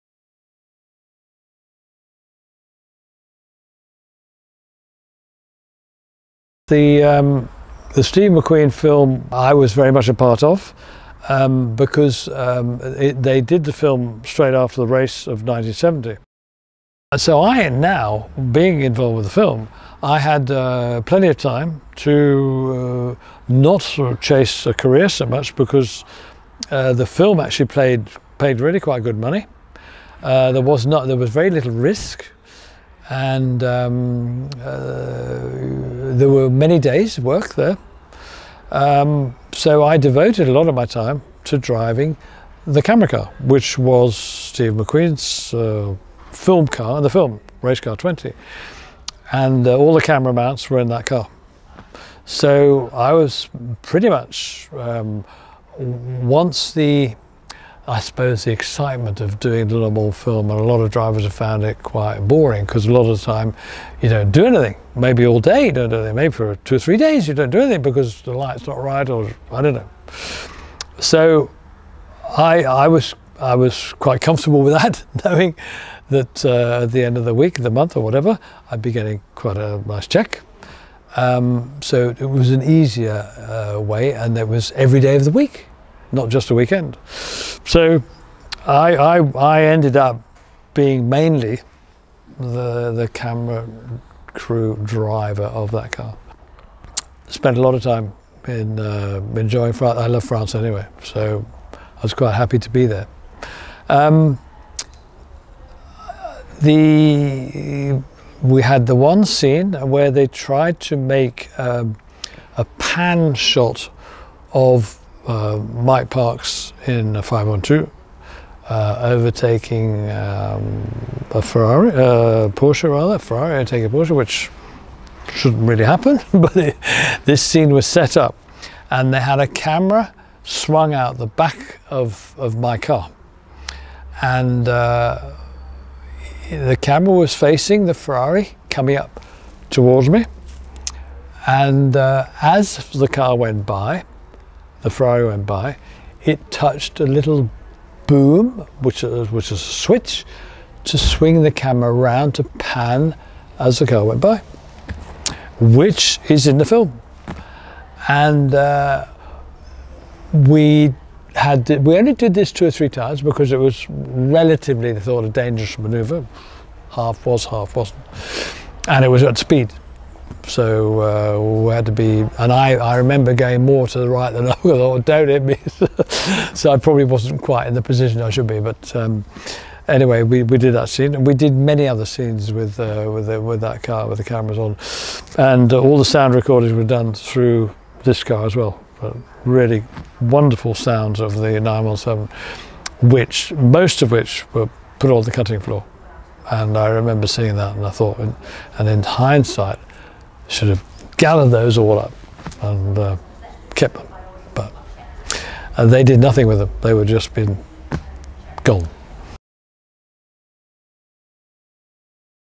Richard Attwood talks about Steve McQueen's Le Mans film Part 1.
Richard Attwood, interviewed here by Porter Press International, recalls his introduction to Steve McQueen’s ‘Le Mans’ film in 1971. He talks about his time as the main Porsche 917 camera car driver, along with the trials and tribulations this work entailed.